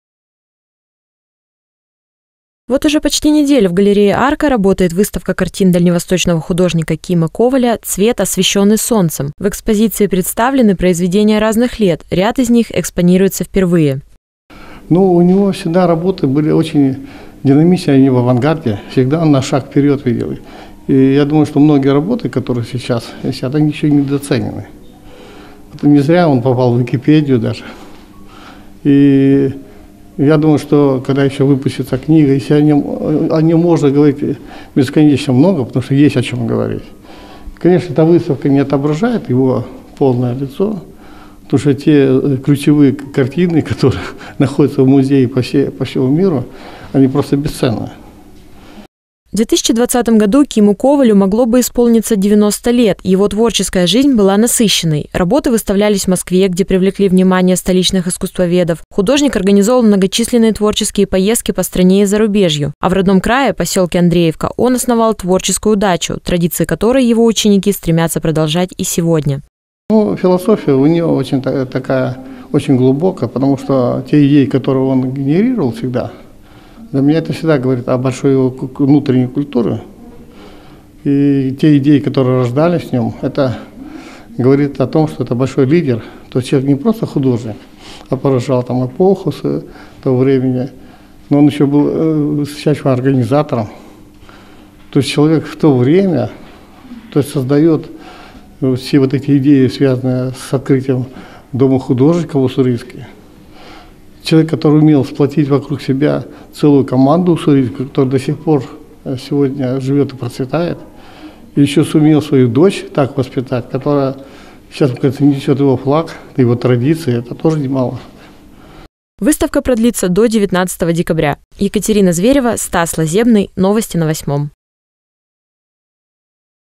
Цвет, освещенный солнцем
С 9 декабря по 19 декабря галерея «Арка» представляет выставку дальневосточного художника Кима Коваля «Цвет, освещенный солнцем». В экспозиции представлены произведения разных лет, ряд из них экспонируется впервые.